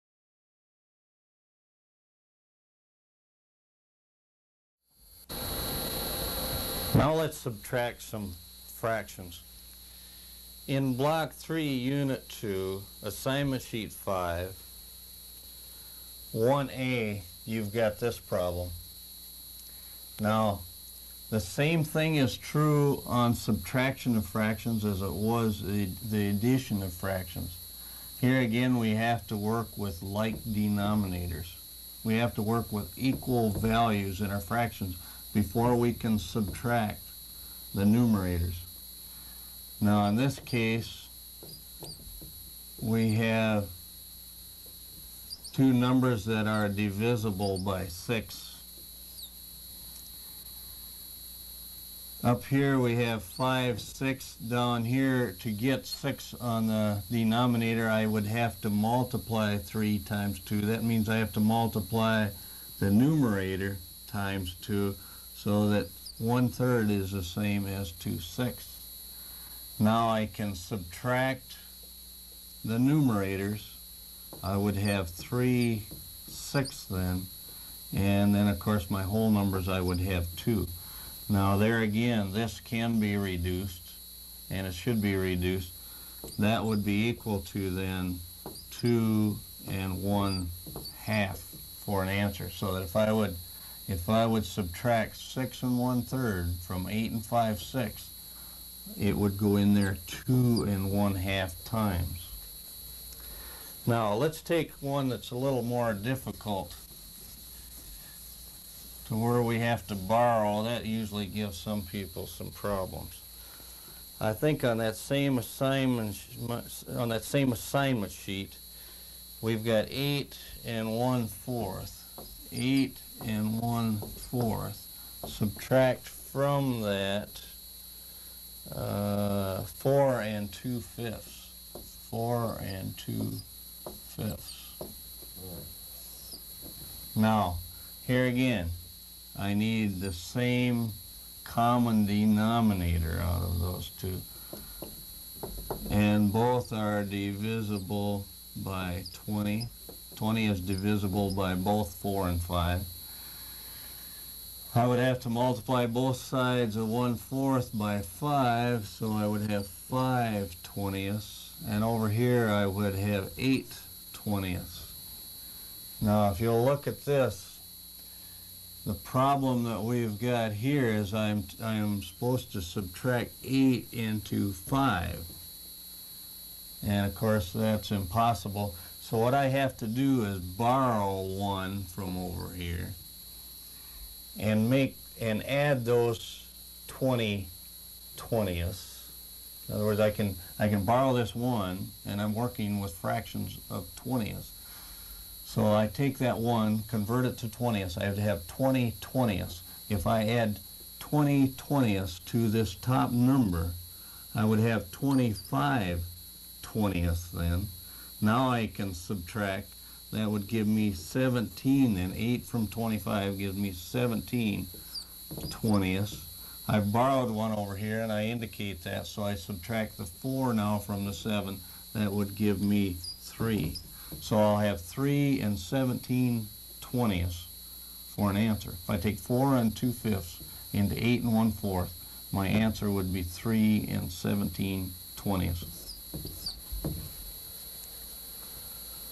Subtracting Fractions
Dennis Merchant video for the electrical math portion of the Merchant Job Training and Safety Program.  Book 1, Section 3, Unit 3-2, "Fractions - Addition/Subtraction."